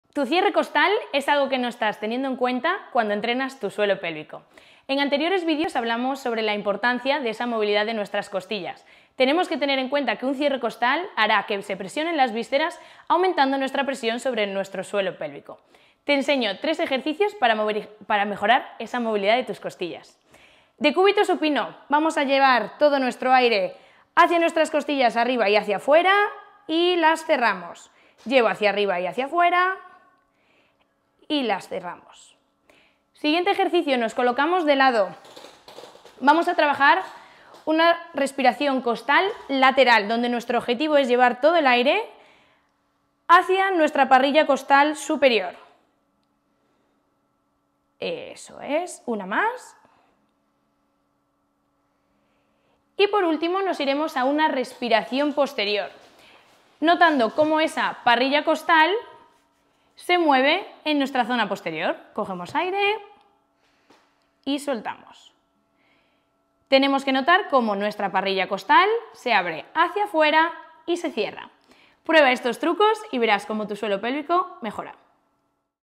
¡Descubre cómo mejorar tu movilidad con estos increíbles ejercicios!
¡Descubre cómo mejorar tu movilidad con estos increíbles ejercicios que te propone Cris! 🤸‍♀️🔥 En videos anteriores, hemos resaltado la importancia de una parrilla costal móvil para gestionar mejor la presión intraabdominal. ⚖️💪 Un cierre costal inadecuado puede aumentar la presión en nuestro suelo pélvico.

Con los tres ejercicios que compartimos en el video, podrás potenciar la movilidad de tu parrilla costal. 🔄👍 Combinados con entrenamientos de pared abdominal y suelo pélvico, ¡podrías experimentar mejoras significativas en tu cavidad abdominopélvica! 🏋️‍♀️🌟 ¿Estás listo/a para transformar tu entrenamiento? ¡Dale play y comienza a mejorar!